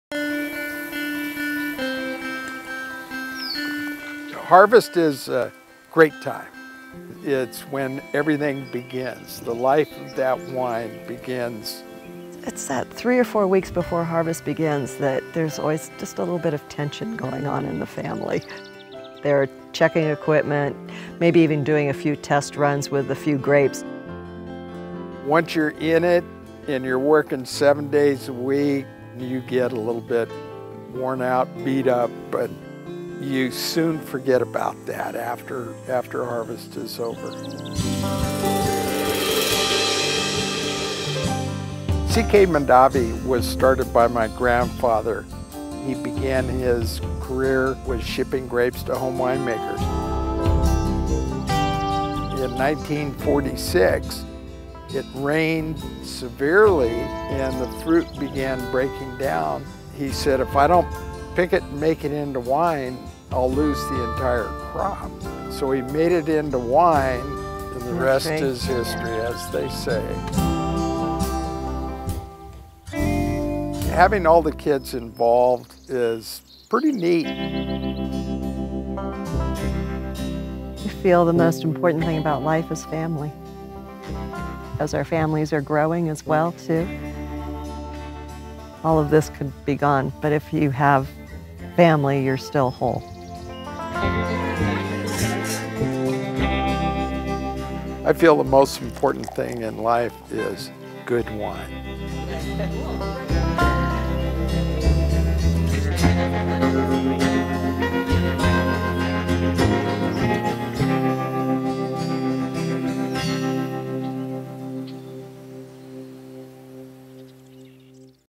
Meet the Family
Meet patriarch and matriarch Marc & Janice Mondavi, Generation Three of the C Mondavi and Family vintners. Get a glimpse behind the scenes at their family (Gen4), their thoughts on winemaking, life philosophies, and their namesake CK Mondavi and Family wines.